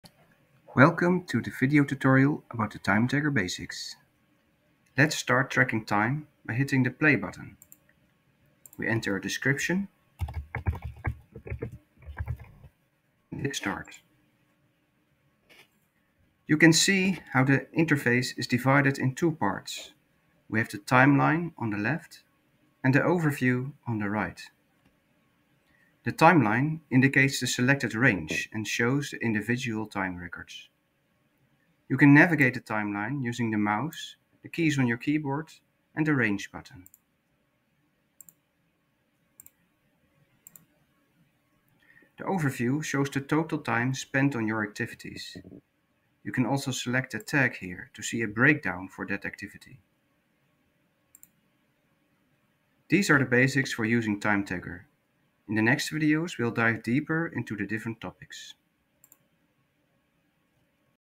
TimeTagger 01 basics
The basics of using TimeTagger

* Starting a record.
  * You can start tracking time by clicking the play button.
  * Fill in a description, and hit start!
* The TimeTagger interface consists of two parts: the timeline on the left, and the overview on the right.
* Timeline.
  * The timeline indicates the selected range, and shows the individual time records.
  * You can navigate the timeline using the mouse, keys, and the range-button.
* Overview.
  * The overview shows the total time spend on your activities.
  * You can also select a tag here to see the breakdown for that tag.
* Summary
  * These are the basics for using TimeTagger.
  * In the next video's we dive deeper into the different topics.